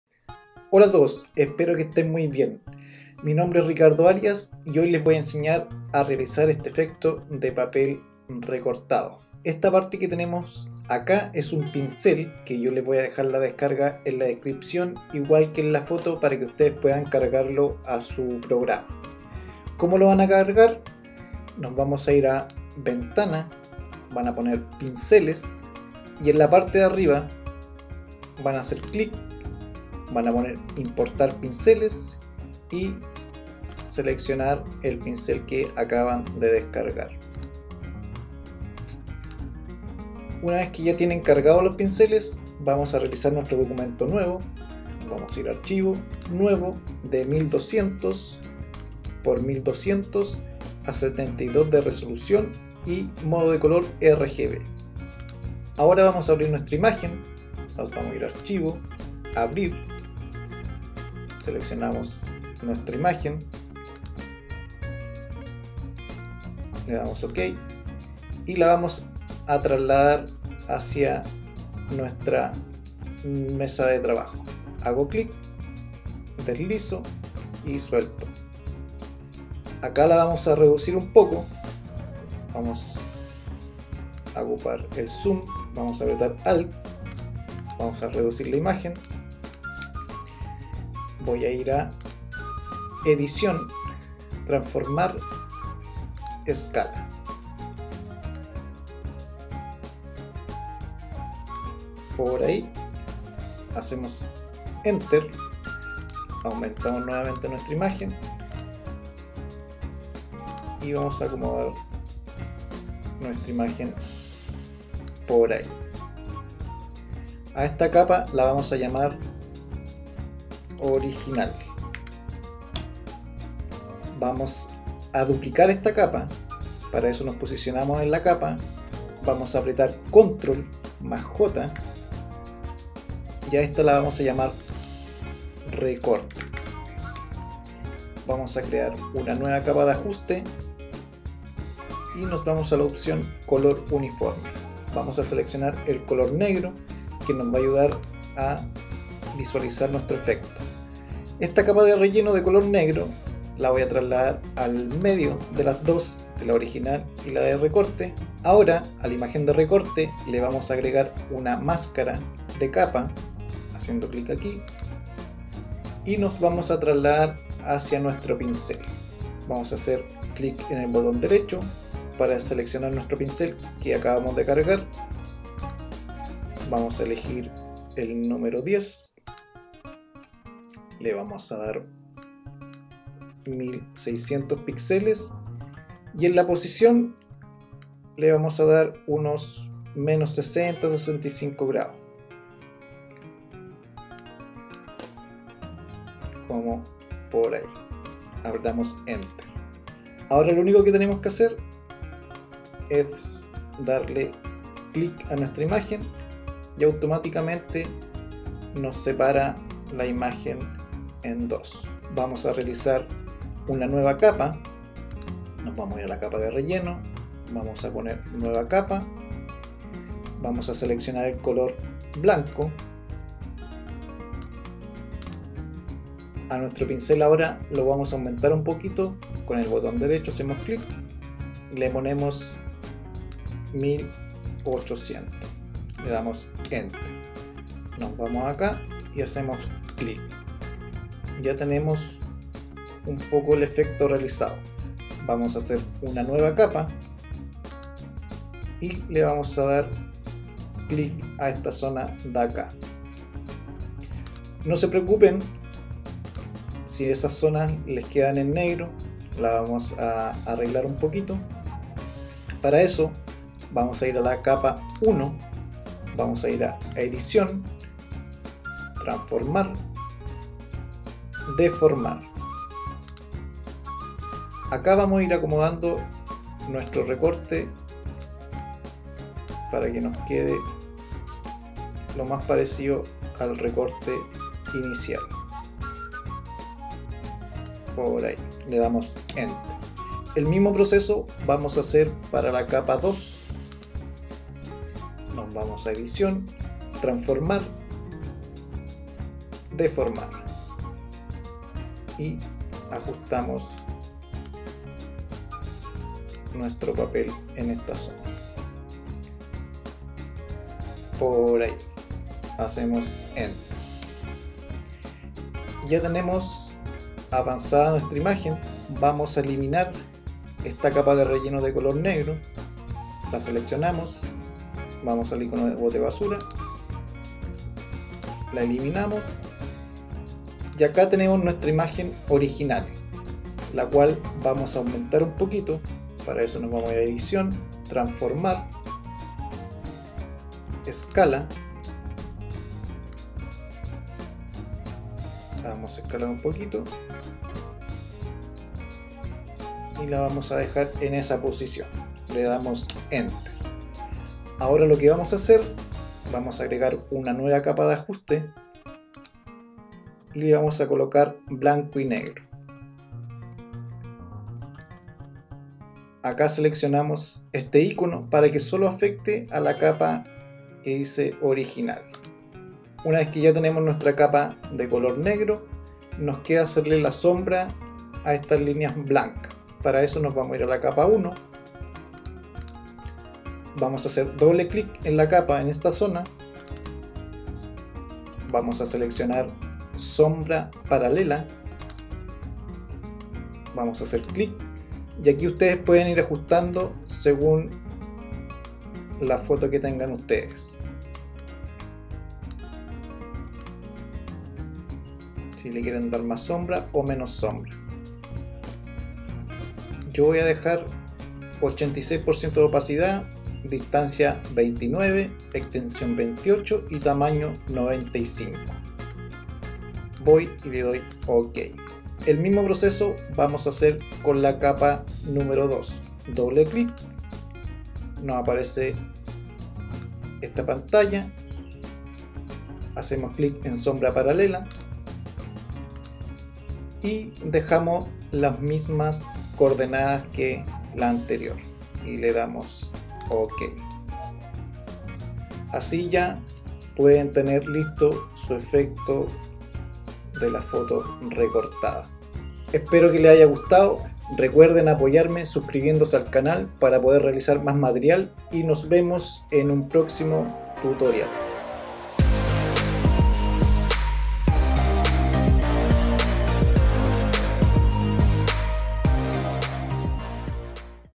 Efecto papel RASGADO con imágenes | Photoshop Tutorial | Torn PAPER effect
Aprende a realizar el efecto de papel rasgado, desgarrado, cortado, roto, destrozado, rajado con imágenes en Adobe Photoshop. ADEMÁS te enseño como agregar pinceles a tu programa.

Acá te dejo los enlaces para que puedas descargar la imagen y pinceles para práctica y realizar tu efecto en photoshop.

Torn paper effect

Efecto de papel rasgado con imágenes